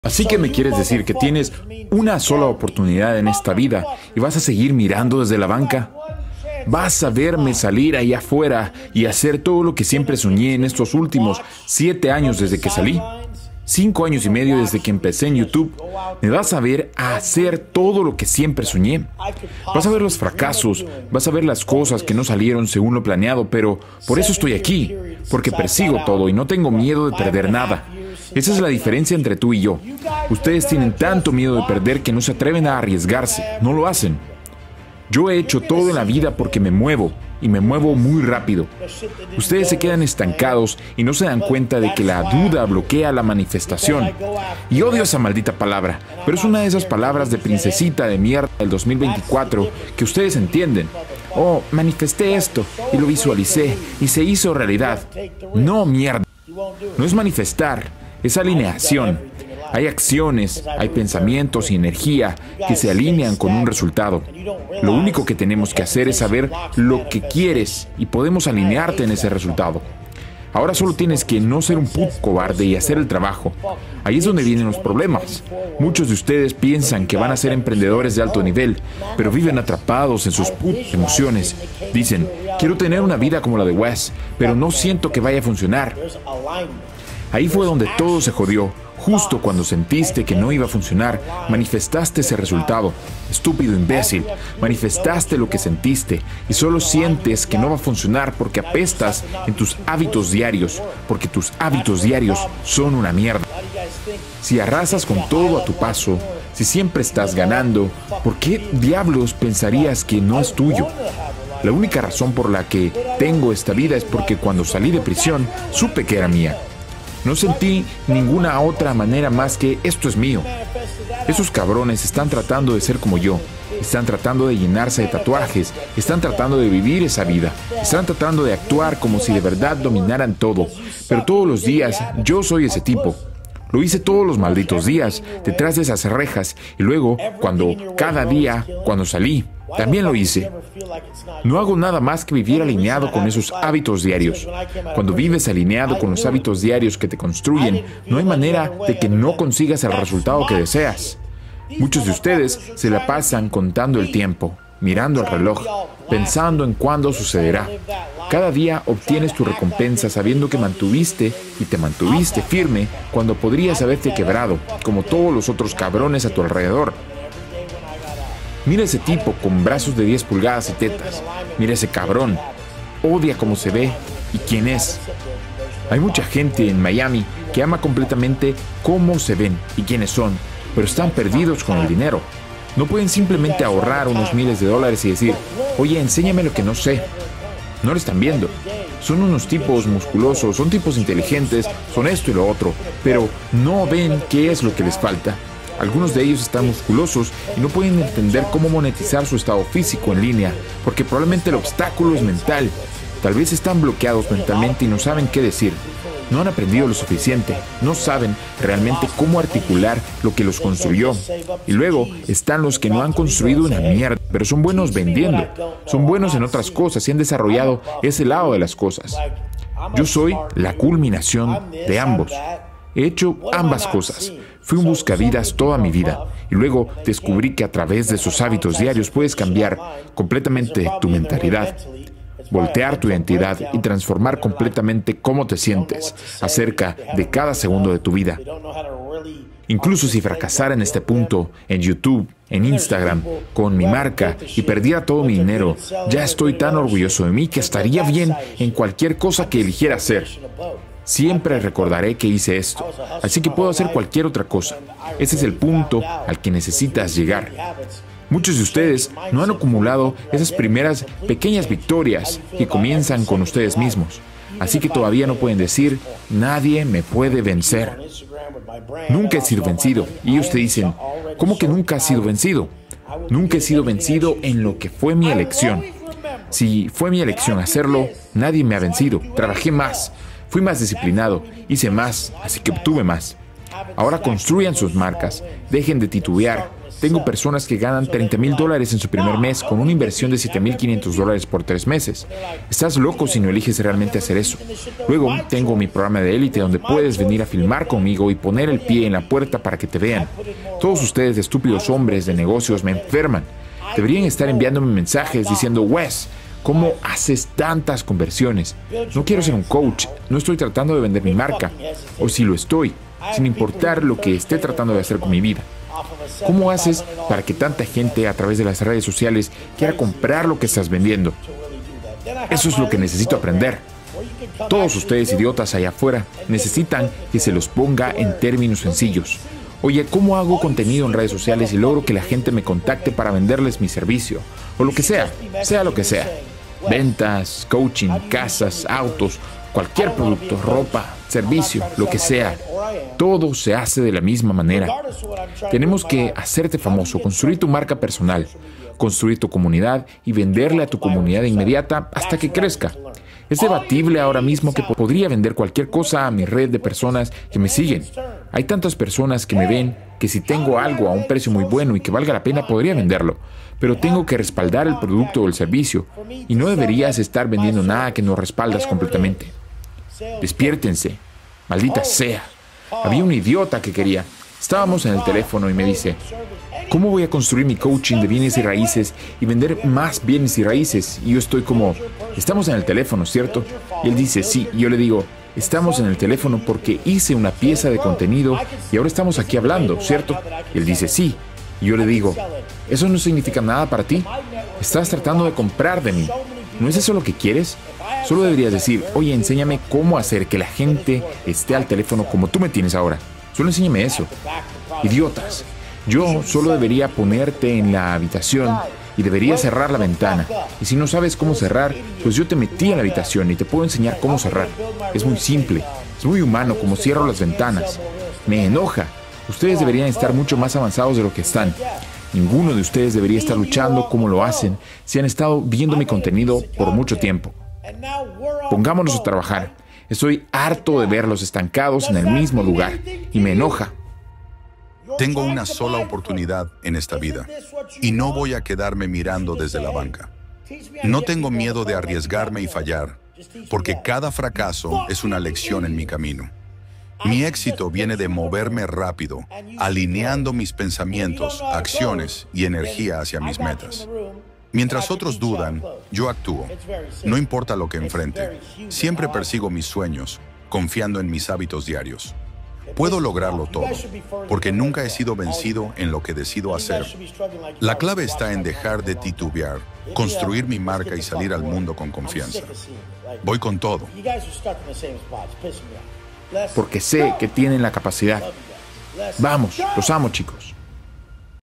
El Mejor y más Poderoso Discurso Motivacional de Wes Watson
Tengo una sola oportunidad en esta vida, y no voy a quedarme mirando desde la banca. No tengo miedo de arriesgarme y fallar, porque cada fracaso es una lección en mi camino. Mi éxito viene de moverme rápido, alineando mis pensamientos, acciones y energía hacia mis metas. Mientras otros dudan, yo actúo. No importa lo que enfrente, siempre persigo mis sueños, confiando en mis hábitos diarios. Puedo lograrlo todo, porque nunca he sido vencido en lo que decido hacer. La clave está en dejar de titubear, construir mi marca y salir al mundo con confianza. ¡Voy con todo!